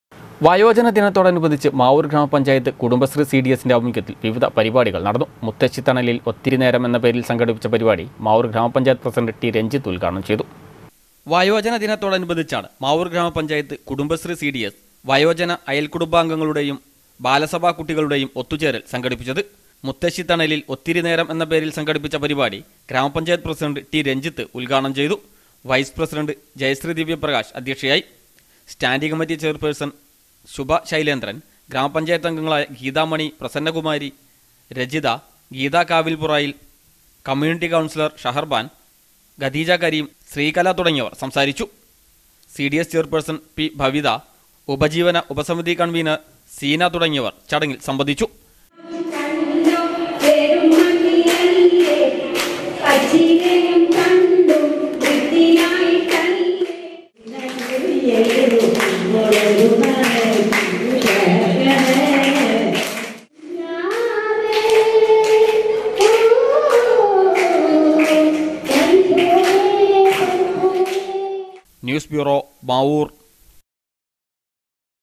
മാവൂർ ഗ്രാമപഞ്ചായത്ത് കുടുംബശ്രീ സി ഡി എസിൻ്റെ ആഭിമുഖ്യത്തിൽ വിവിധ പരിപാടികൾ നടന്നു
വയോജന ദിനത്തോടനുബന്ധിച്ച് മാവൂർ ഗ്രാമപഞ്ചായത്ത് കുടുംബശ്രീ സി ഡി എസിൻ്റെ ആഭിമുഖ്യത്തിൽ വിവിധ പരിപാടികൾ നടന്നു. മുത്തശ്ശി തണലിൽ ഒത്തിരി നേരം എന്ന പേരിൽ സംഘടിപ്പിച്ച പരിപാടി മാവൂർ ഗ്രാമപഞ്ചായത്ത് പ്രസിഡൻ്റ് ടി. രൻജിത്ത് ഉദ്ഘാടനം ചെയ്തു